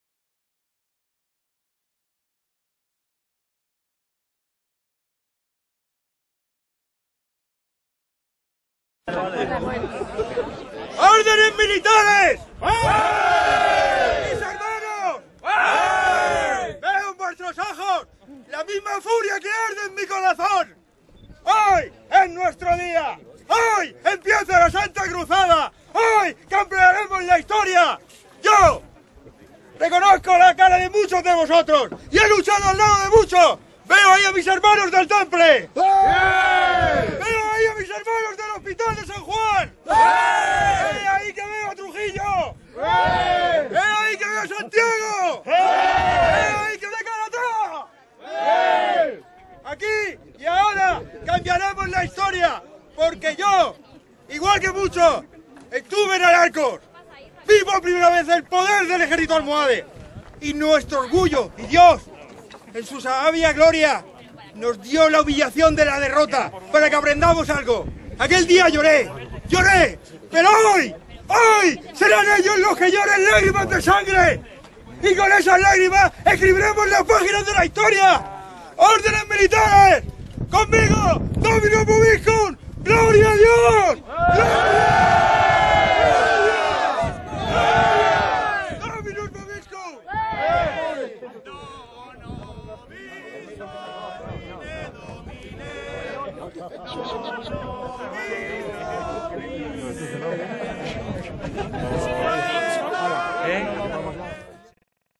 Discurso de Pedro Arias. Maestre de Santiago. Navas de Tolosa 2012.
Discurso de Pedro Arias Maestre de la Orden de Santiago. Moriría junto a los otros Maestres de las Órdenes Militares en la batalla de la Navas de Tolosa, donde las Órdenes Militares a pesar de la victoria sufrieron grandes bajas.